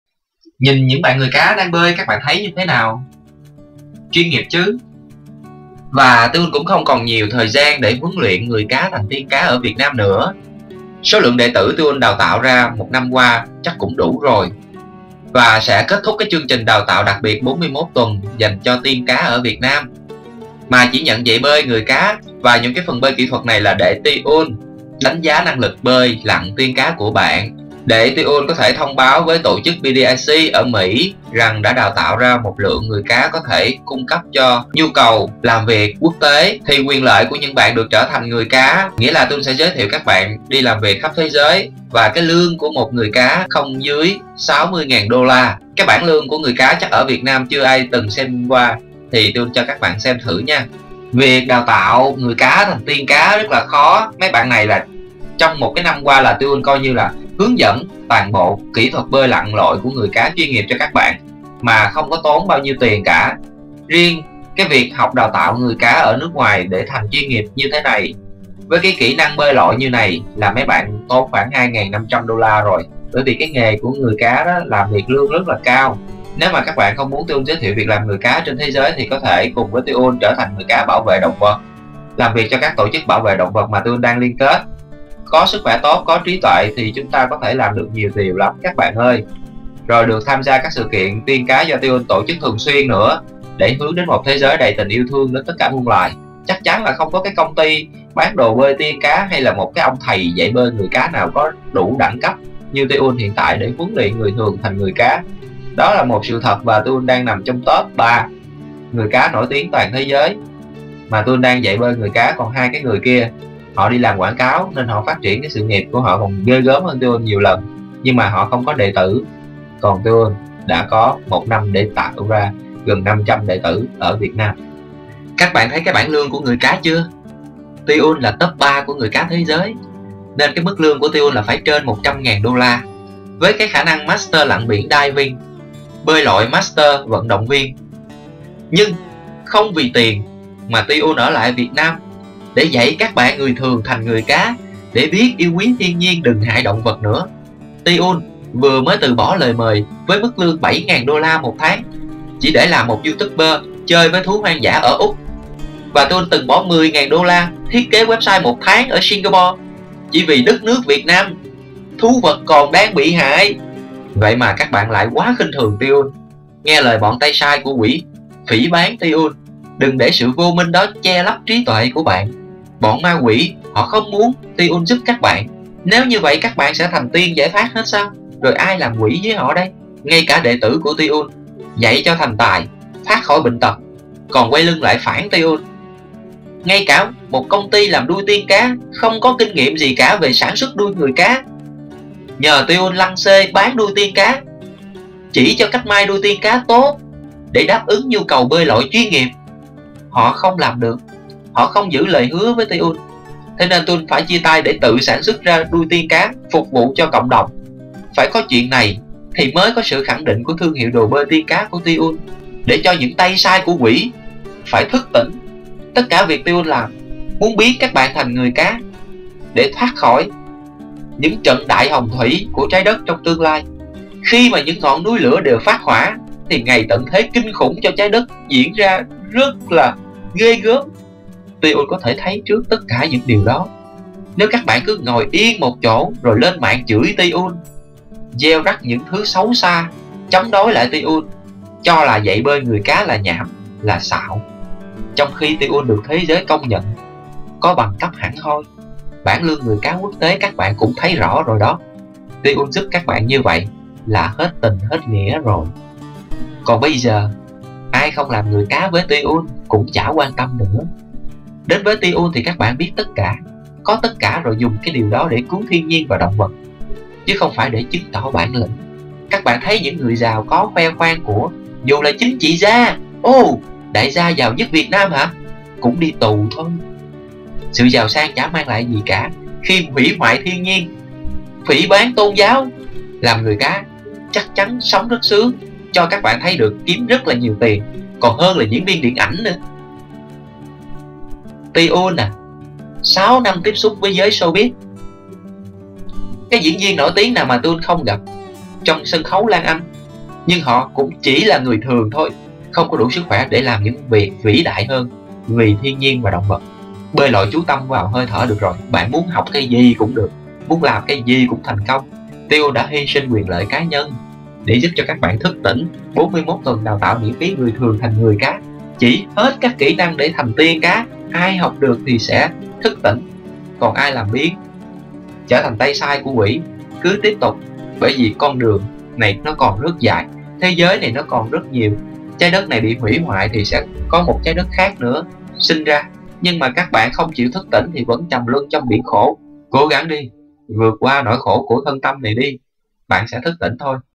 Thank God Mermaids Are Here | Thành Người Cá Đừng Làm Tay Sai Của Quỷ
You thought you was fulfilling your own dream, but the real magic was in making children's dreams come true. For those who don't know, mermaiding is a thing. Donning a fin and living your best merlife is more than a lifestyle: It's also a profession, whether you work in a bar, as entertainment for events, or to help promote ocean conservation.

Càng muốn thành tiên càng dễ bị cám dỗ, vì phải đủ lực mới có thể vượt qua các cảnh giới của ma quỷ thần. Hiểu được điều đó, nên Tiun sẽ không bao giờ trách sự vô minh của các bạn đệ tử người cá, nhưng nếu mọi người không sớm thức tỉnh để cứu thiên nhiên và động vật thì sẽ xảy ra tận thế trái đất khiến tất cả muôn loài đều bị đọa địa ngục. Tiun không còn đủ năng lực và thời gian để huấn luyện người thường thành tiên cá nữa. Vì vậy sẽ kết thúc chương trình đào tạo đặc biệt 41 tuần của tiên cá ở Việt Nam như dự kiến. Mà chỉ nhận dạy bơi người cá bình thường mà thôi, còn ai có duyên thức tỉnh học hỏi theo 41 tuần đào tạo đó, chắc chắn sẽ thành tiên cá biết tất cả. Còn quyền lợi của những bạn người cá đến với Tiun bây giờ chắc chắn sẽ được trở thành người cá bảo vệ động vật cho các tổ chức mà tiun đang liên kết, cùng nhau tham gia các sự kiện liên quan để hướng đến một thế giới đầy tình yêu thương đến tất cả muôn loài.